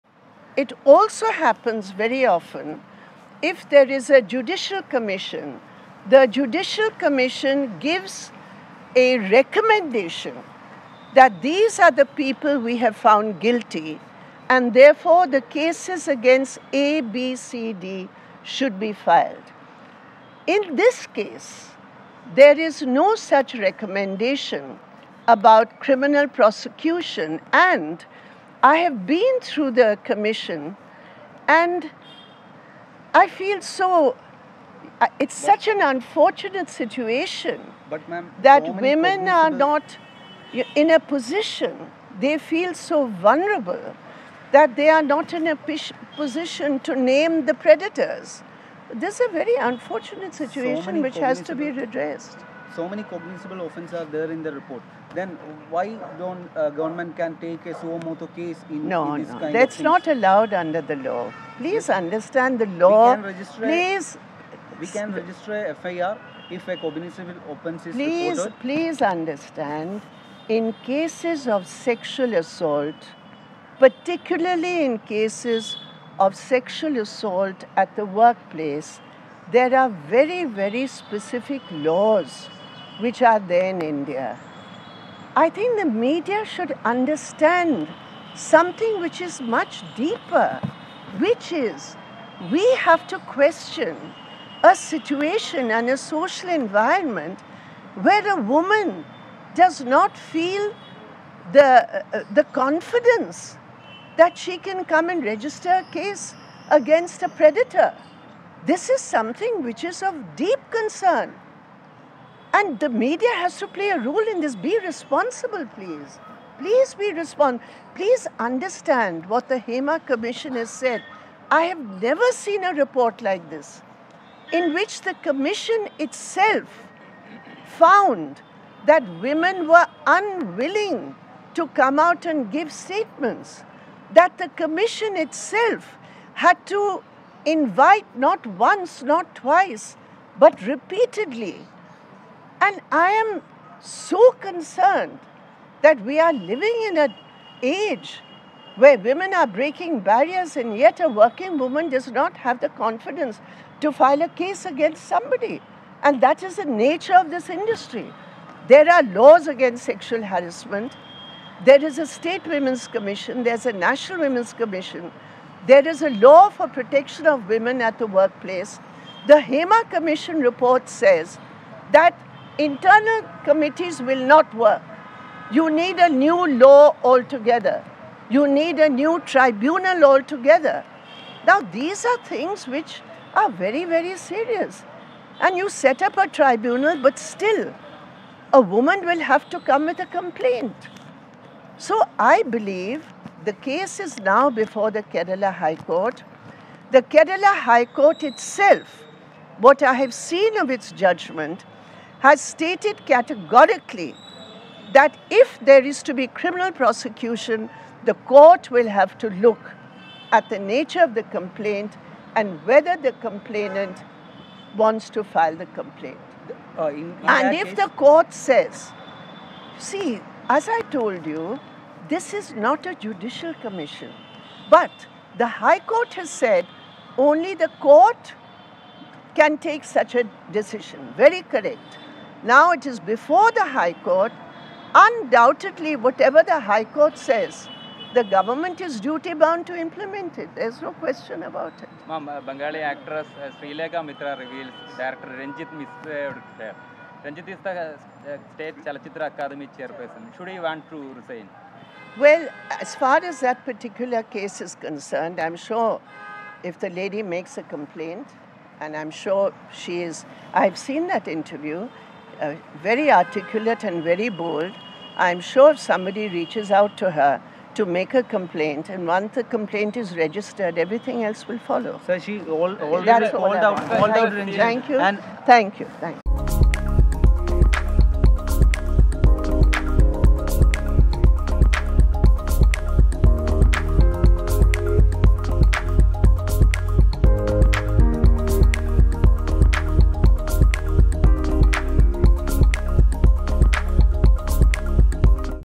'ശ്രീലേഖ മിത്ര പറഞ്ഞത് പരാതിയായി രേഖപ്പെടുത്തണം' ബൃന്ദ കാരാട്ട്
'What Srilekha Mitra said should be recorded as a complaint', Brinda Karat